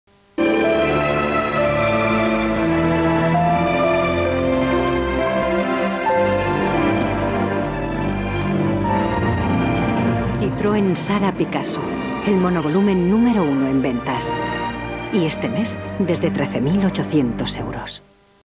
Anuncio Citroën Xsara Picasso - 2003
Citroën Xsara Picasso, el monovolumen número uno en ventas. Y este mes, desde 13.800 euros.